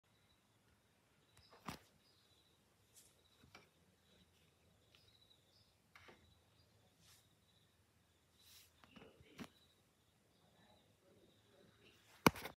How to put a banana on your internet router
A simple tutorial to place banana on router